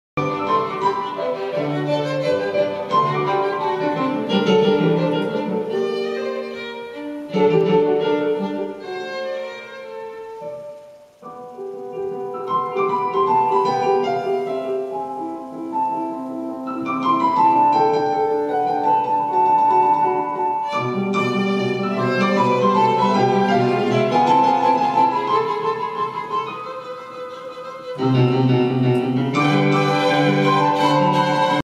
NEMPACSarahGlennBenXia.MTS
NEMPAC Scholarship Concert - February 27, 2010 - Sarah Glenn & Ben Xia